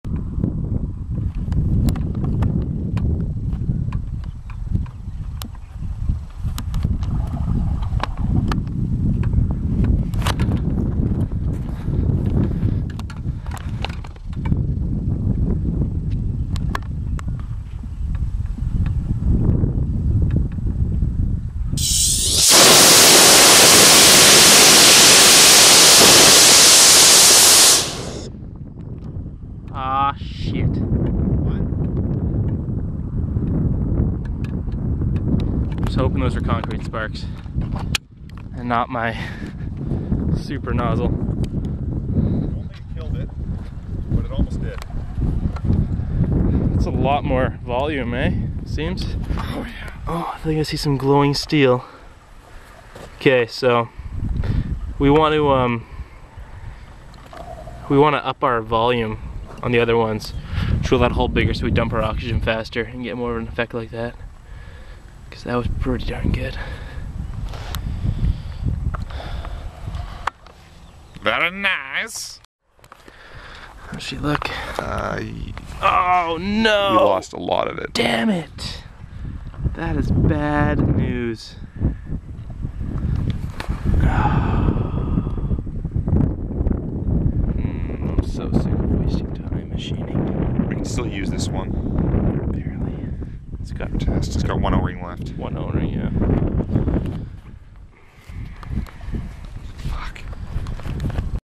#14 Hybrid rocket motor static test, burning up the nozzle holder.
Stupidly decided to reuse the already eroded concrete insert from #13, with an increased flow of O2. May still be able to save the insert but we will  be down to one sealing O-ring instead of two. Back to the lathe..